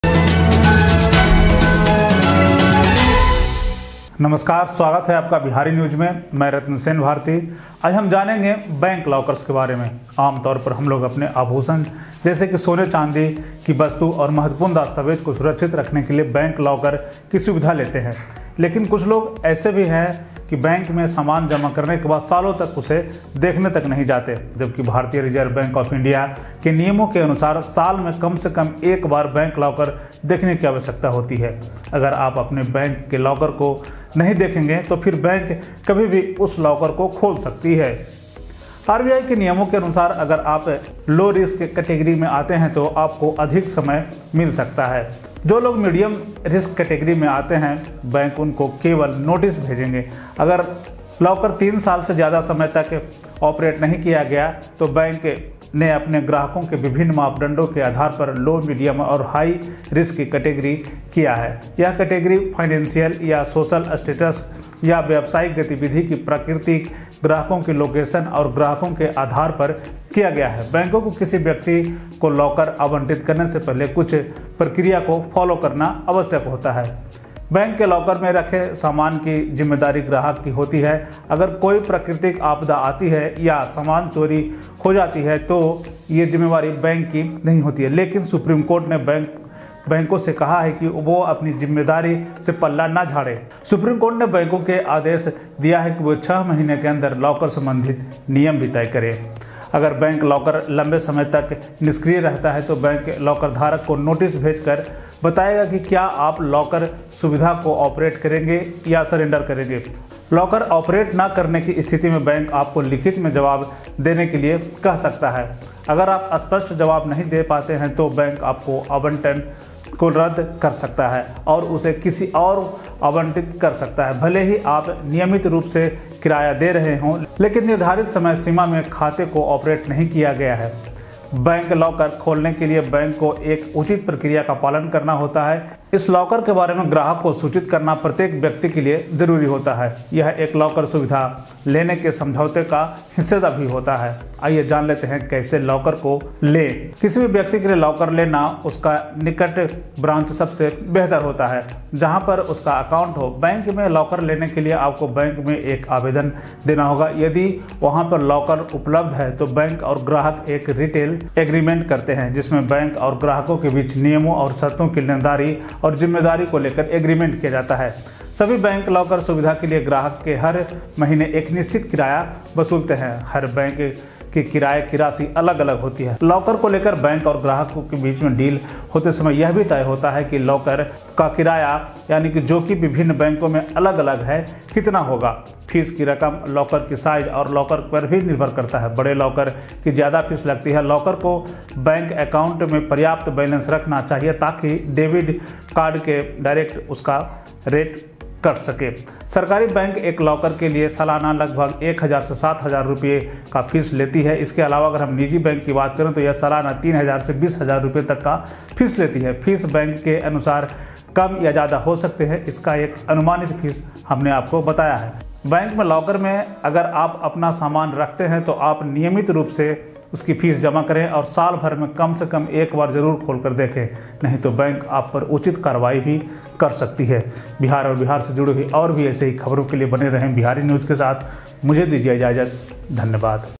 बैंक लॉकर के बारे में जान लें जरूरी बात, नहीं तो पड़ेगा पछताना
Usually, we take the facility of bank locker to keep our jewelery such as gold and silver items and important documents safe. But some people are such that after depositing the goods in the bank, they do not even go to see it for years.

Whereas according to the rules of the Reserve Bank of India, it is necessary to see a bank locker at least once a year. If you will not see the locker of your bank, then the bank can open that locker at any time. If the bank locker is inactive for a long time, the bank will send a notice to the locker holder and tell if you will operate the locker facility or surrender. In the event of the locker not operating, the bank may ask you to give a reply in writing.